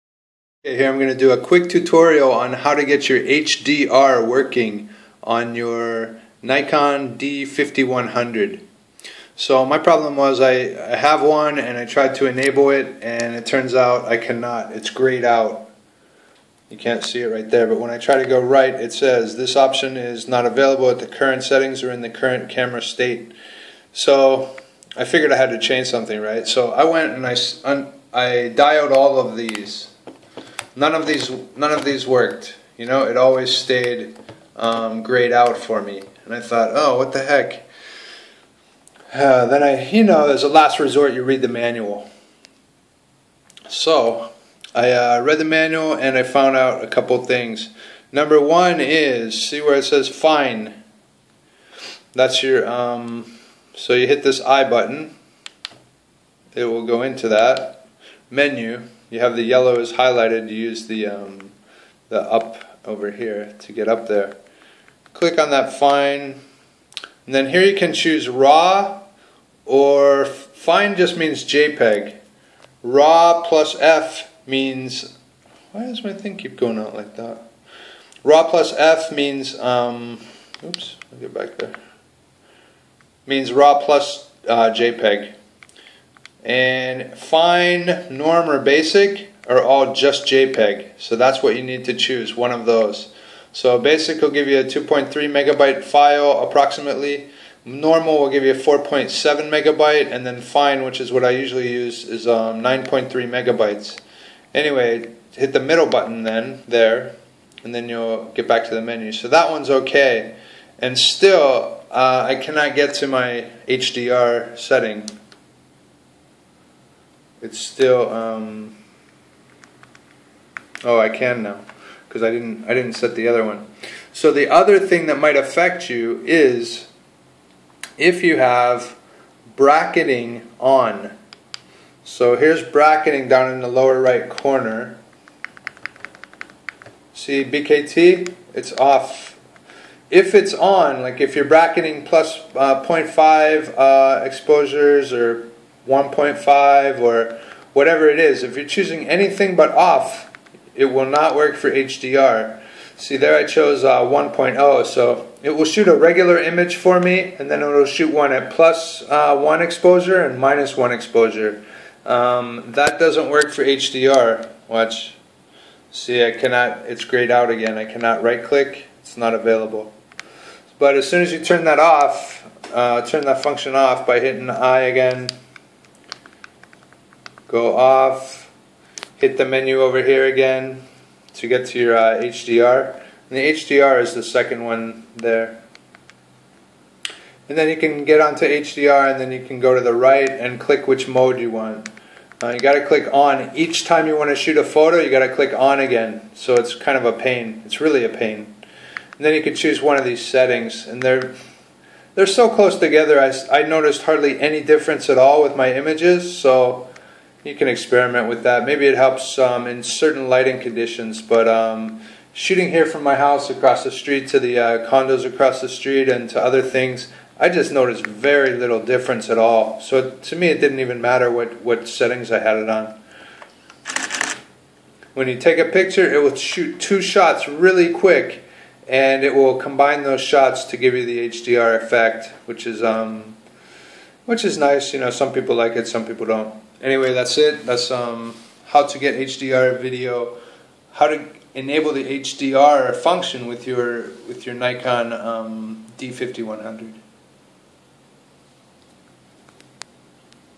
HDR Instructions - Nikon D5100 Settings (How To Use High-Dynamic-Range)
Nikon D5100 DSLR cameras have HDR (High-Dynamic-Range) functionality built-in and ready to use. How to set this up is easy, but not very intuitive. After I cycled through the dial on the top I decided to read the frickin' manual and figure it out. Turns out there are a few things that affect your ability to take HDR photographs on the Nikon D5100, and the settings must be just right or you won't have the menu item enabled.

Specifically You Must Not Have These Functions Enabled:

1. Bracketing.
2. RAW data images. You MUST shoot JPG files - whether Fine, Norm, or Basic. If you choose RAW or RAW+F, RAW+N, etc, you will not be able to reach the HDR menu - it will be grayed out.
3. Any of the easy dial choices on the top right dial except M (Manual shutter and aperture), A (Aperture priority), S (Shutter priority), or P (Program). You must have your dial set on one of these 4 options. The others will cancel the HDR menu item and you won't be able to choose it.

When you shoot HDR mode, the camera will take two images of different exposure levels and it will merge them to automatically create your HDR image. You MUST set the HDR to "ON" each time you want to take an HDR image. Kind of dumb, right?

The additional settings within HDR mode are, in my experience, very similar to each other. I noticed very little difference between any of the images taken with different settings. It might even be best to let the camera choose the settings by leaving it at AUTO.

If you want to know more about HDR photography, there are tutorials that help you do it in Photoshop and other graphics programs. In general, the more shots you take at different exposure levels, the greater the dynamic range of the image, and the more amazing the possibilities.

* 640+ Videos about Thailand.